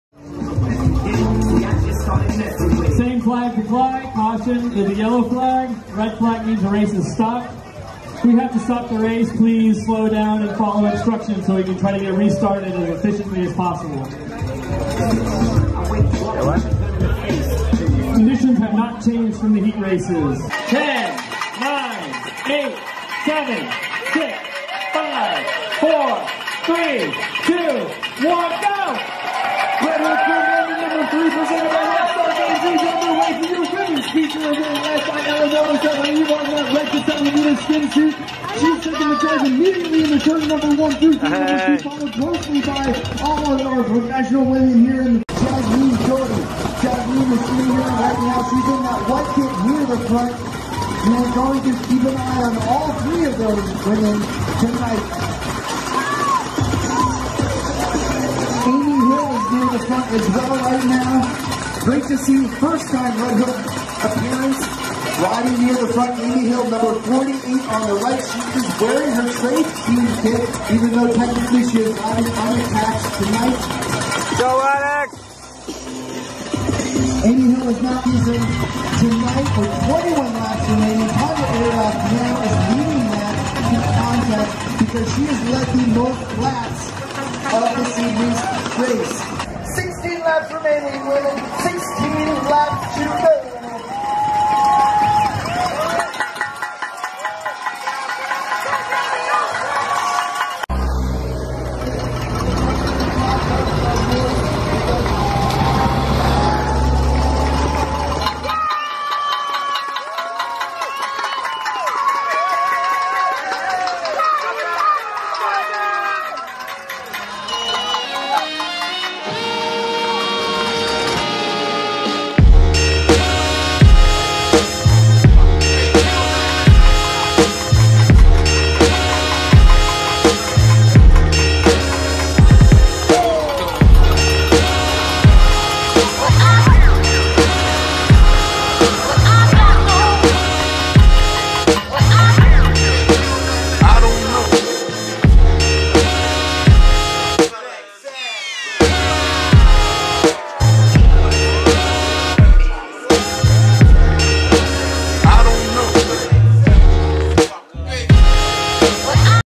Red Hook Crit London Women's final Race
Some clips from the Women's final race @ the Red Hook Crit London 3